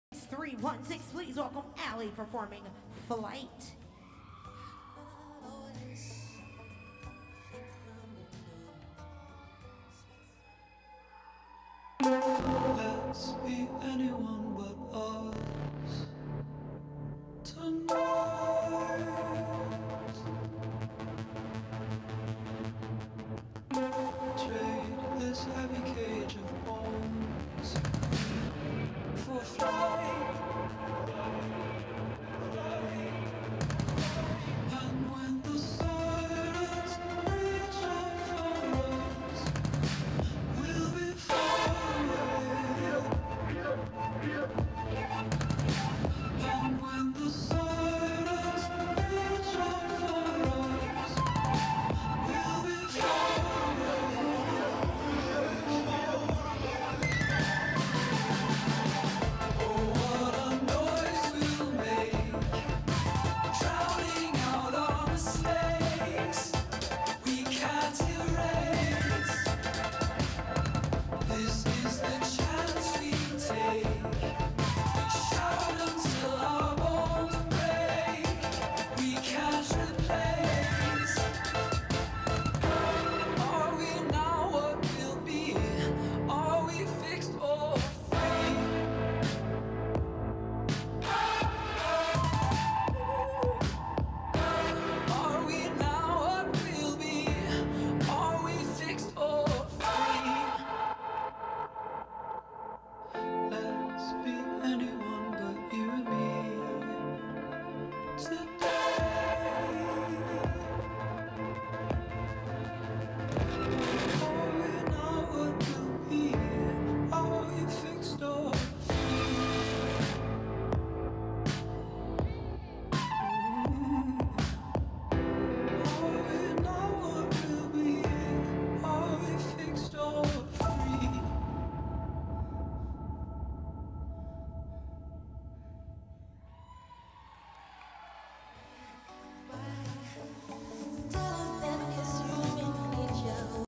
Alli Chu - Flight
Senior • Solo • American • Open • Age 15
ORANGE COUNTY PERFORMING ARTS ACADEMY
Hall of Fame Ontario 2018